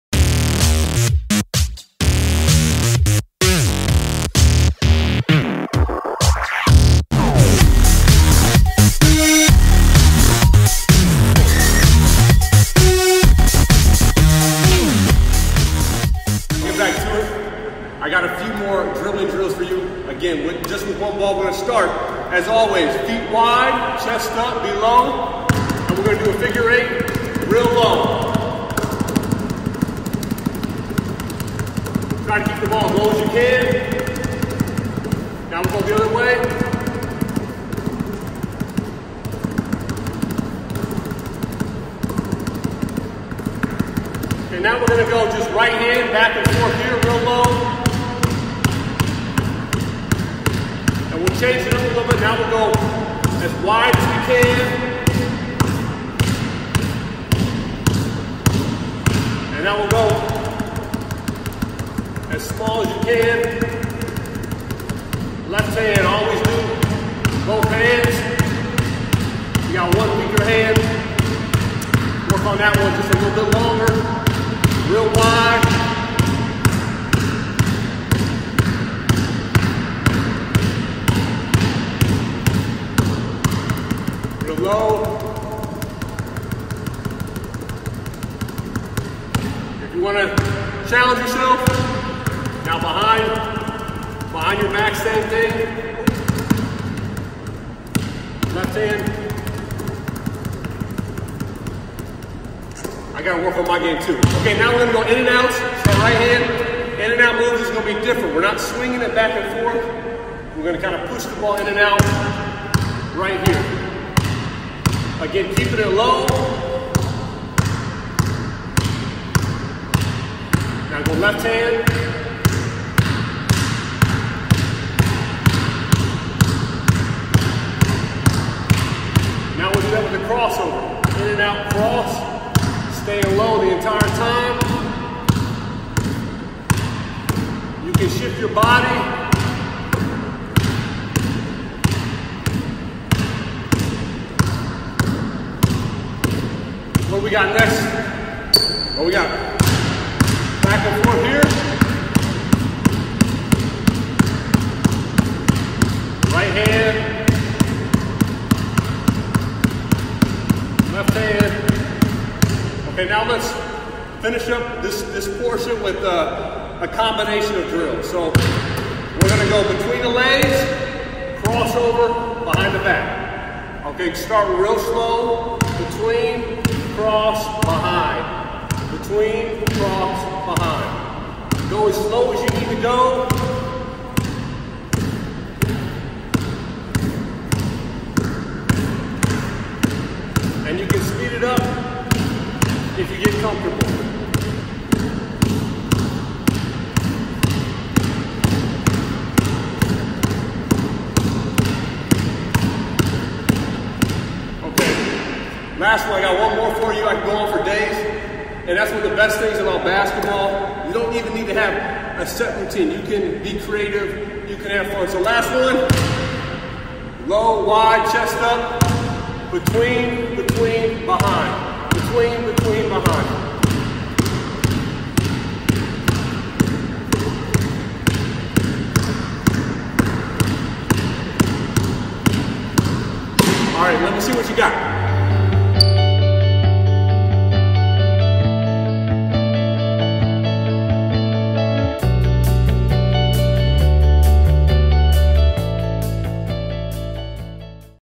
Basketball Skills Development #2 with Trent Meacham
Join former Illini and Basketball Pro, Trent Meacham, at the Stephens Family YMCA to work on your basketball skills. This second video in our ongoing features additional single-ball drills prepping you and/or your child for our "Beat the Pro" Challenge.